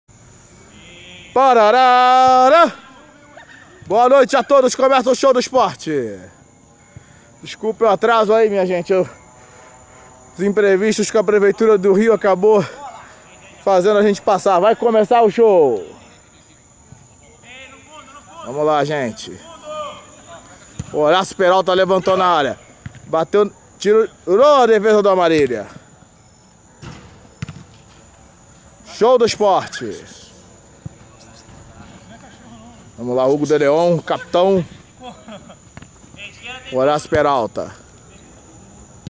Gavião do Aterro (Show do Esporte) - Verdugo x Amarilla em 04/12/2012 (parte 01)
Verdugo

1-Raphael(Mondragón)

2-Alcides(Di Stéfano/Lima Duarte)

3-João Hallak(Hugo de León)

4-Claudinho(Válter Minhoca)

5-Temporão(Horácio Peralta)

6-Gustavo Azougue(Émerson Sheik)

7-Afonso JR(Gentile)

Amarilla

8-Nego(Felipe)

9-Tinho(Júnior César)

10-Gustavinho Mont Blanc(Ricardinho)

11-Dani Bastos(Bebeto)

12-PH(Adilson Batista)

13-Dedé(Sávio)

14-Lobão(Keirrison)